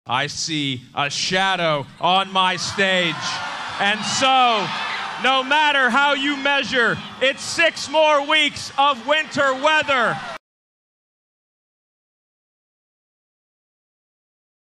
Will Punxsutawney Phil See His Shadow?
Legend has it that if America's favorite marmot sees his shadow, then there are six more weeks of winter ahead. If not, spring will arrive early.